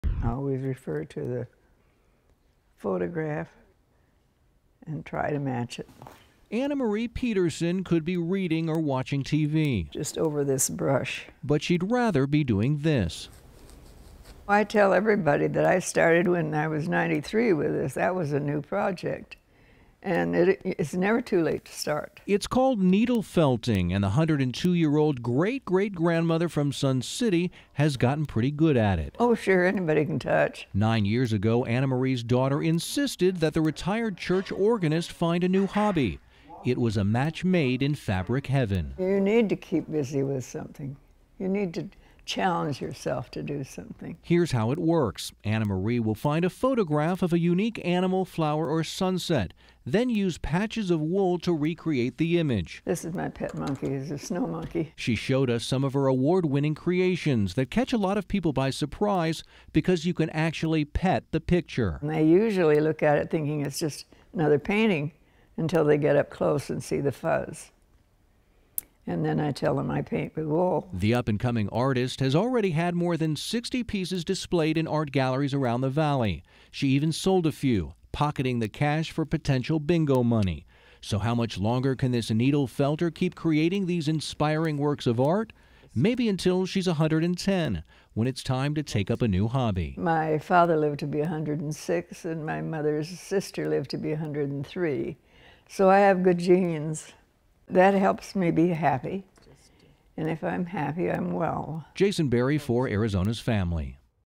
VIDEO: Great-great-grandmother creates award-winning work with wool
A 102-year-old woman shows you can learn something new as she is now an award-winning artist with her creations made out of wool.